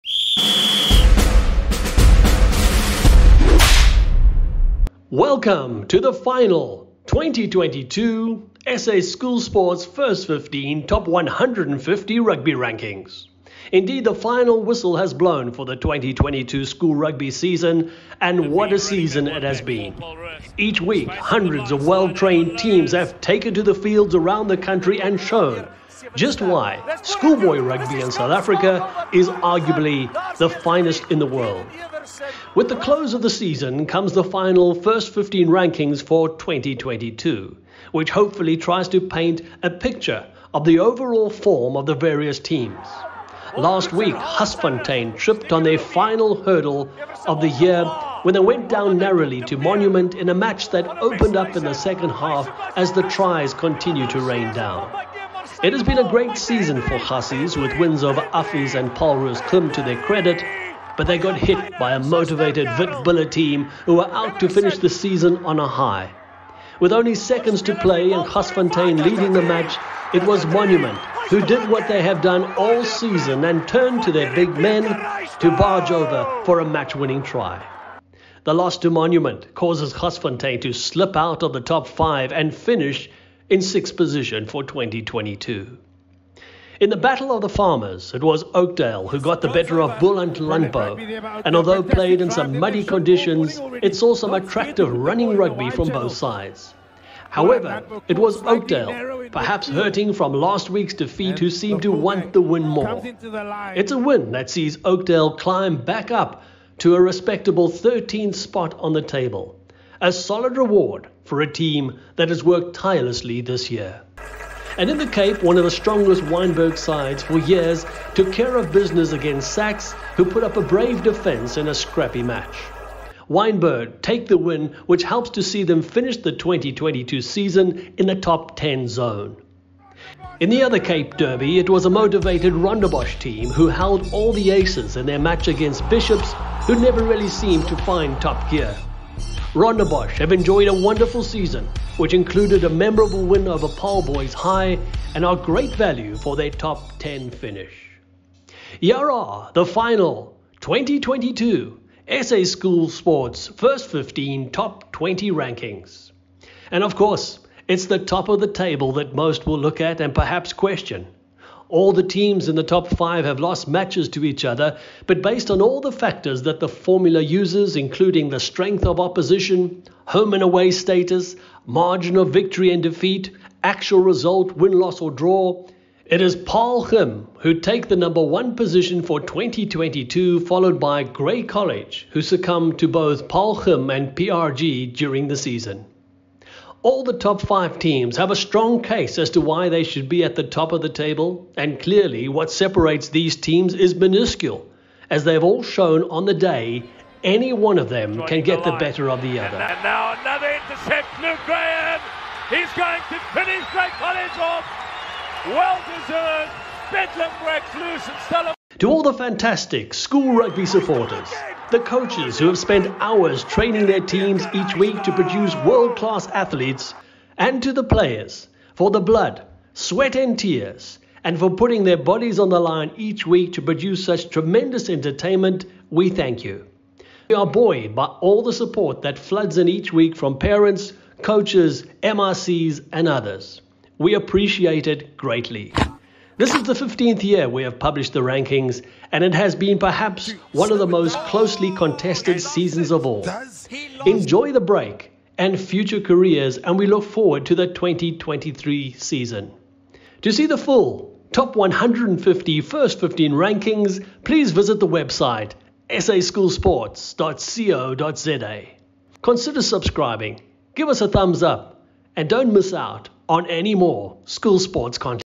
First XV School Rankings (29/08/2022)
The School rugby season is over and here is the final First XV School Rugby Rankings for 2022.  Take a look at who the top rugby schools are for this year.
The Ranking formula takes into consideration several key factors including: Strength of opposition, Home/Away Status, Margin of Victory/Defeat, Points scored and others.
Download the Super Sport Schools app
Alex Combrink (Rondebosch footage)
Jan van Zyl photography